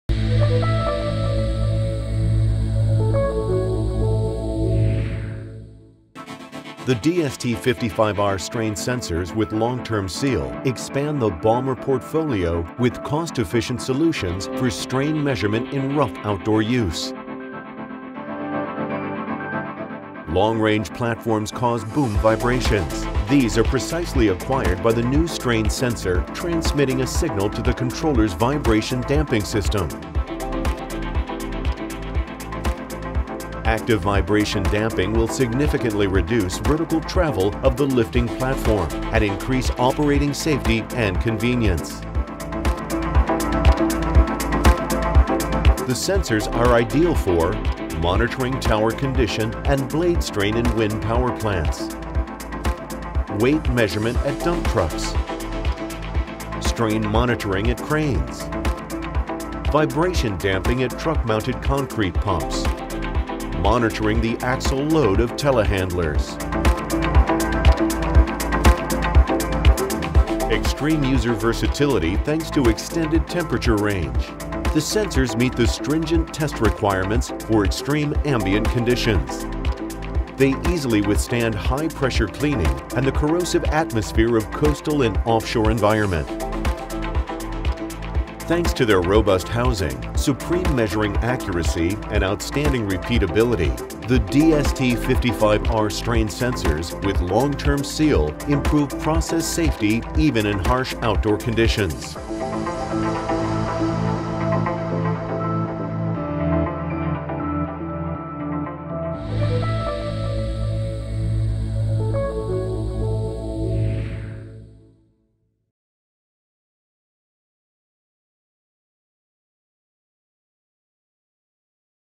Baumer | Strain sensors DST55R | Strain sensors for mobile machines and outdoor applications
The robust strain sensors are based on DMS technology. The strain sensors with long-term seal, offering a cost-effective solution for measuring in tough outdoor conditions. They are particularly suitable for industrial applications in the outdoor environment such as mobile automation. Factory calibration to elongation allows straightforward installation without having to calibrate with a separate force sensor.